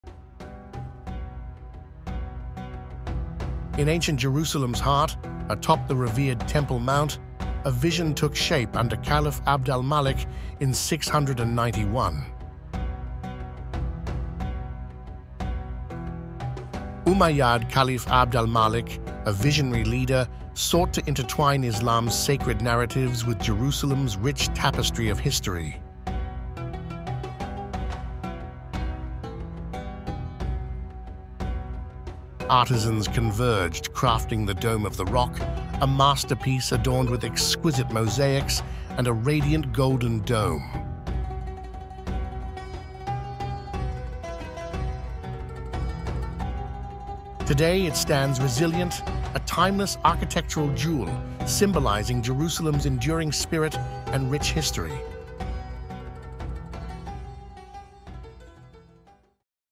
✨The Dome of the Rock✨🕌
Dome of the Rock: A Timeless Beacon of Jerusalem
Embark on a visual journey through the epochs to the revered Temple Mount in ancient Jerusalem, where the illustrious Dome of the Rock was conceived and brought to life. Under the visionary leadership of Umayyad Caliph Abd al-Malik, artisans from diverse lands converged to craft a masterpiece that seamlessly wove together the sacred narratives of Islam with the city's rich historical tapestry. Today, the Dome stands as a resilient, timeless architectural marvel, symbolizing the enduring spirit and rich history of Jerusalem. Join us as we traverse through time, exploring the inception, construction, and eternal significance of this iconic landmark.